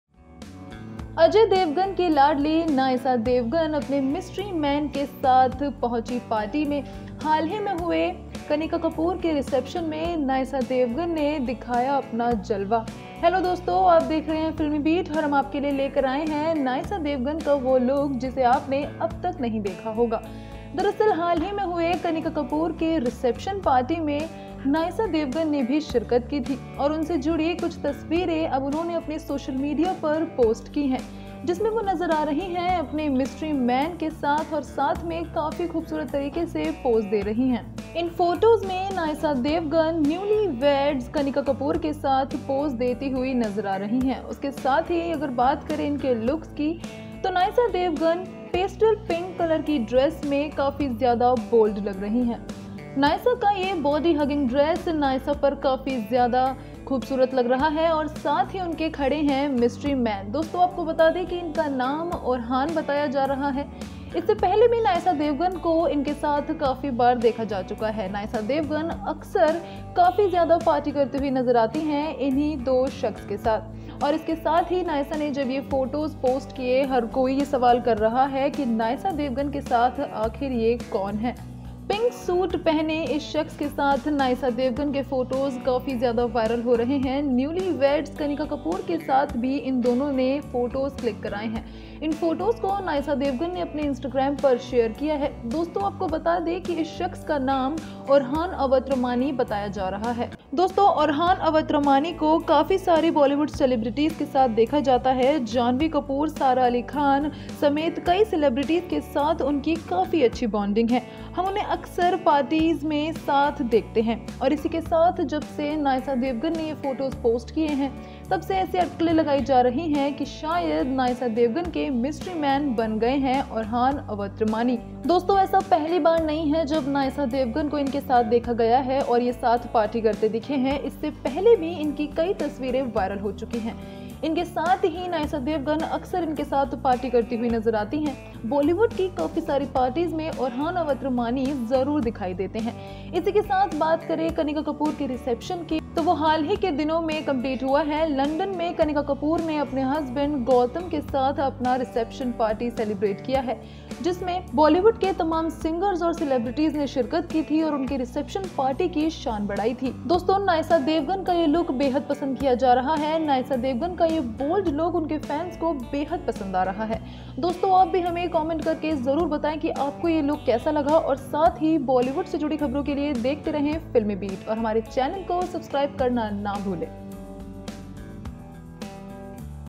Nysa Devgan का bold लुक आया सामने, Kanika Kapoor के reception से हुई तस्वीरें लीक | FilmBeat
Nysa Devgan's Bold look goes Viral. Ajay Devgan's daughter Poses with Her friend at kanika kapoor reception party. Kanika Kapoor reception photos gets leaked | watch Video to know